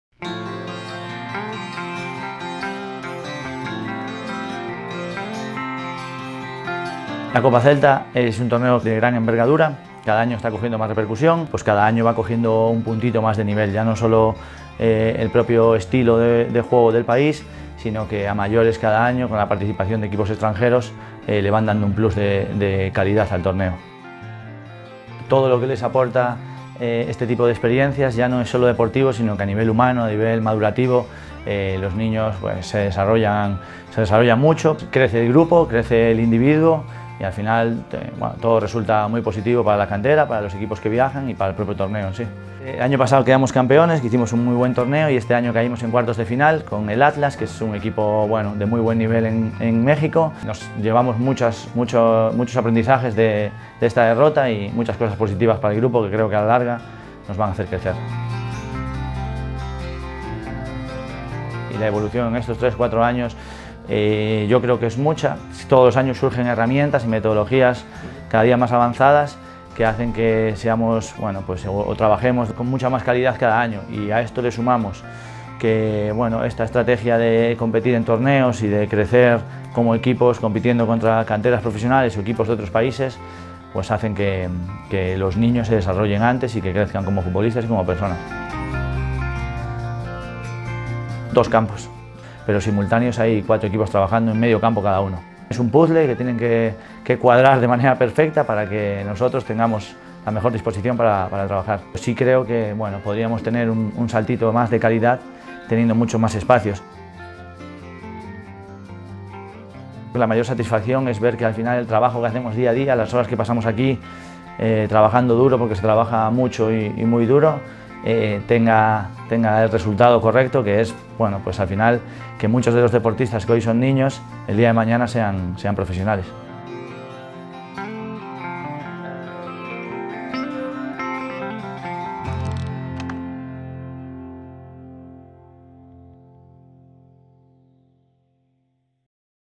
Coñecendo A Canteira | Celta Infantil A + Copa Celta Campeche
Celta Media entrevista ao adestrador do Celta Infantil A, equipo que recentemente participou na Copa Celta organizada pola escola celeste en Campeche (México).